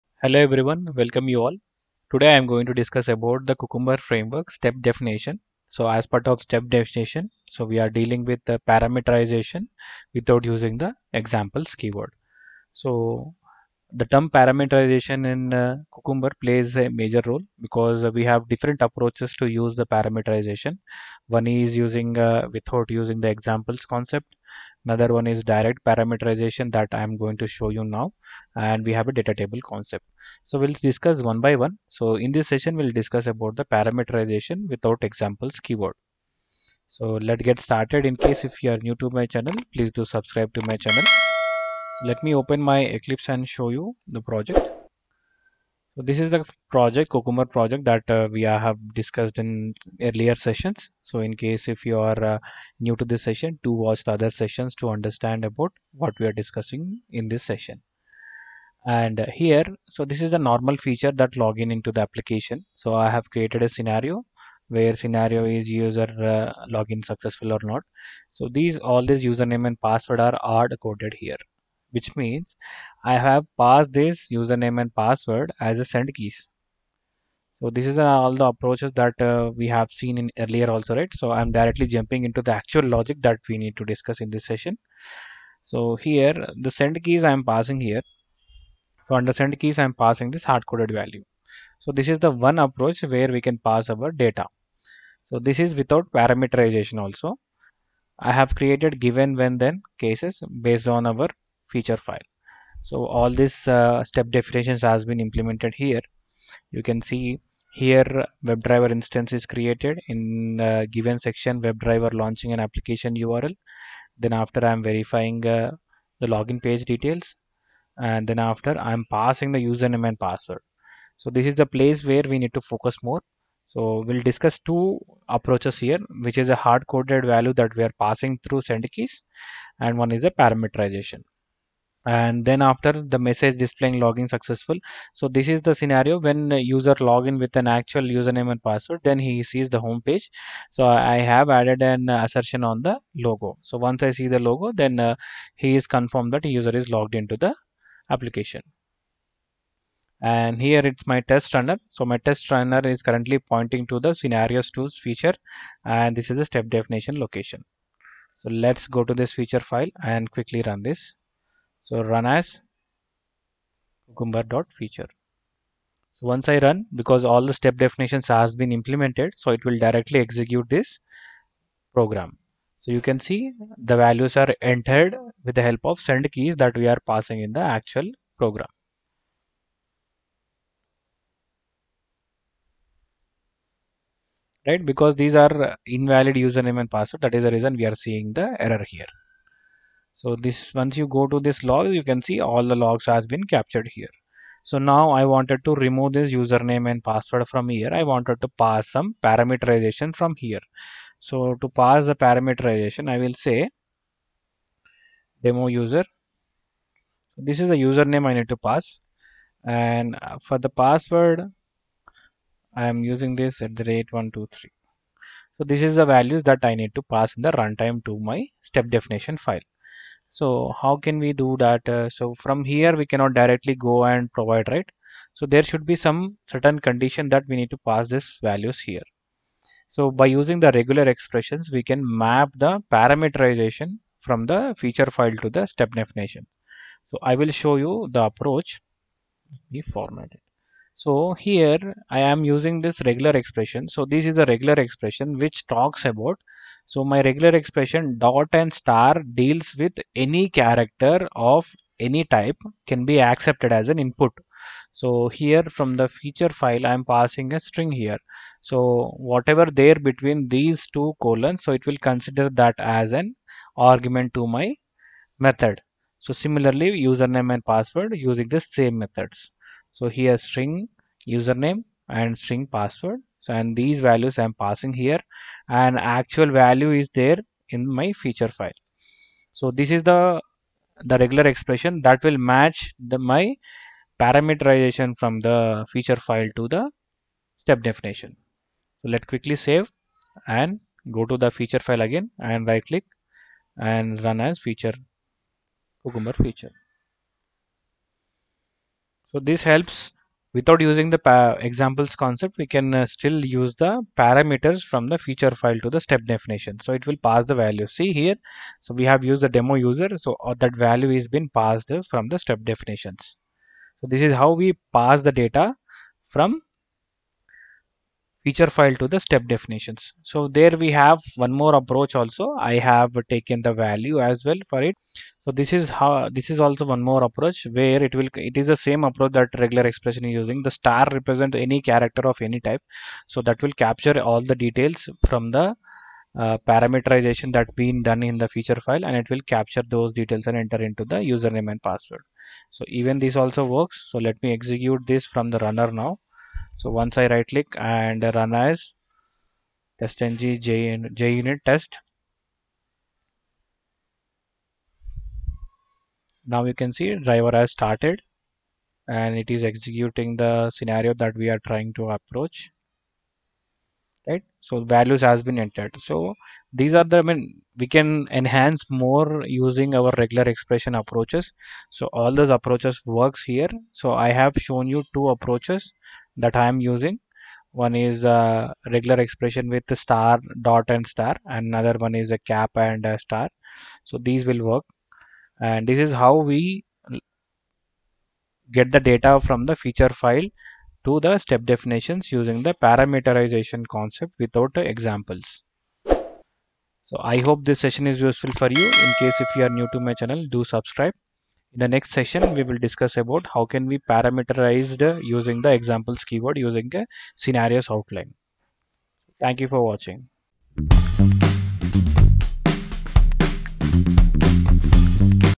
#5 Cucumber Test Case Parameterization without using Example Keyword | SDET
Hello

In this tutorial, I will explain you how to parameterization without example keyword in cucumber

Step by step guide for the setup of Cucumber Framework Setup

Step 4) Create Maven Project in Eclipse IDE

Step 6) Create Class for Code and Follow My instructions

Hope you enjoyed in learning

Check out other Series offered by Suresh SDET Automation